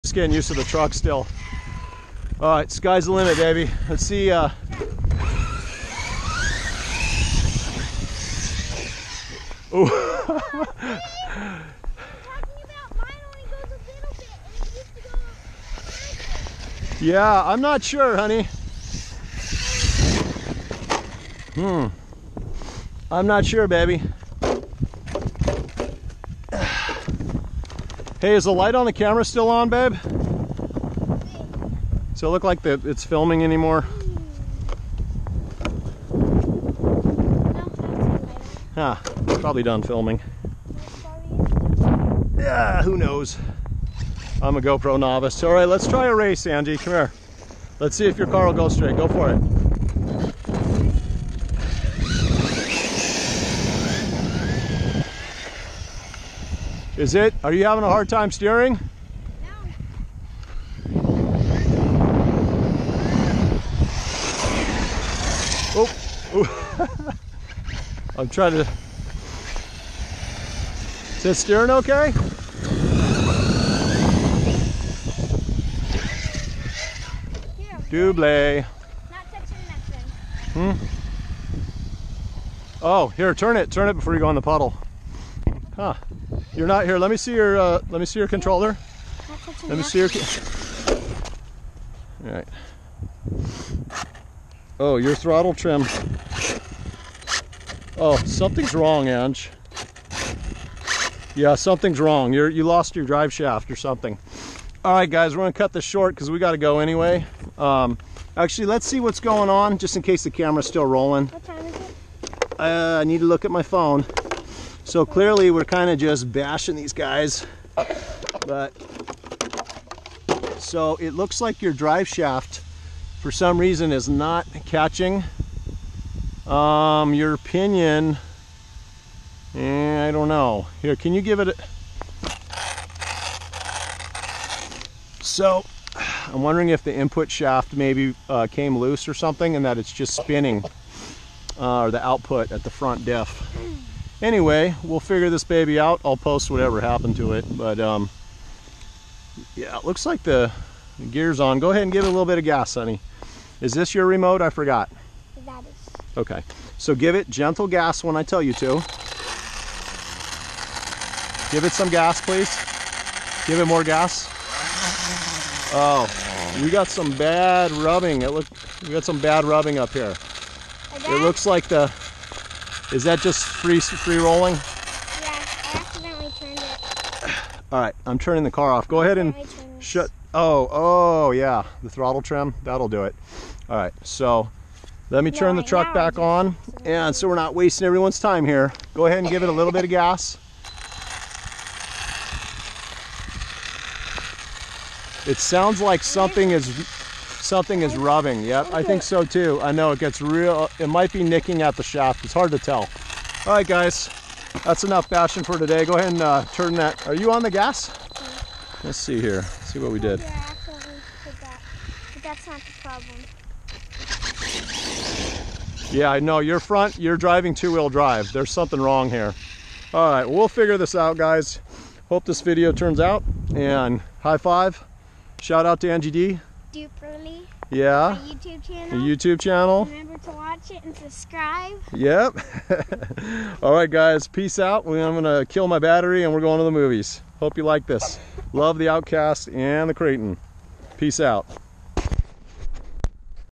Arrma Outcast 6s vs Kraton Beat Down 2/2
Ending part of hard family bash with Angie D driving Kraton on 4s for first time. Both rigs using dBoot Backflips tires. Solved Kraton front drivetrain issue. After cracking open front diff to check, my initial thoughts were correct. Front driveshaft diff output grub screw had backed out and was slipping on main input shaft. Grub wouldn't turn so I thought it was tight. Fortunately, output fell off shaft while tinkering with front diff. Had to use copious heat with gun to break grub loose and reinstall correctly onto main input shaft. PS replaced broken Outcast bumper with Tbone and stock mini bumper and screws from Typhon. Fits great on Outcast Buggy platform. Will bash test of course!